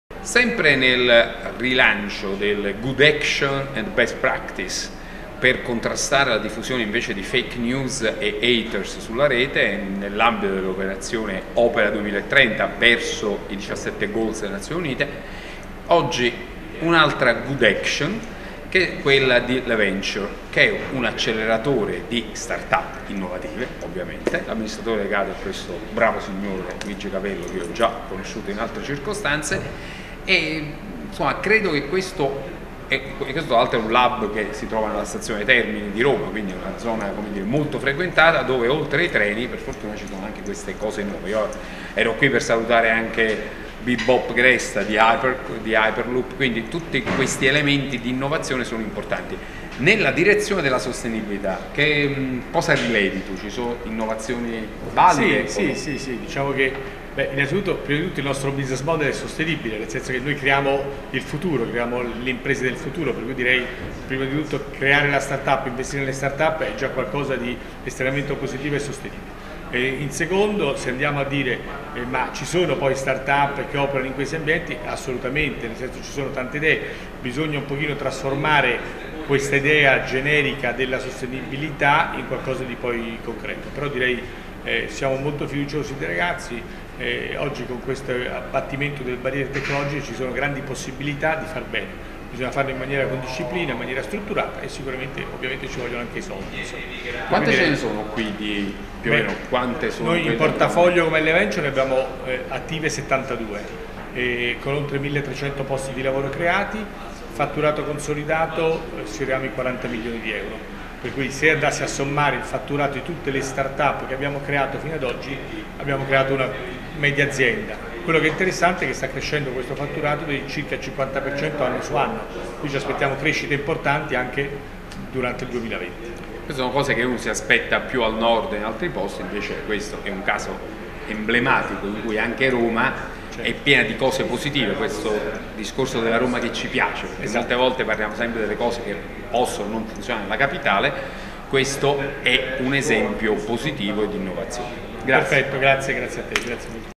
Alfonso Pecoraro Scanio con Luigi Capello (Hub LVenture Group / LUISS EnLabs)
Il Presidente della Fondazione UniVerde Alfonso Pecoraro Scanio con Luigi Capello, CEO di LVenture Group, nel grande Hub di Roma Termini che ospita tanti innovatori digitali.
Attraverso l'acceleratore LUISS EnLabs, LVEnture Group è una realtà che investe in startup digitali, attualmente oltre 70 nel portafoglio, e ne accelera il processo di sviluppo dal prototipo al mercato, dando vita a progetti che diventano impresa e creano posti di lavoro.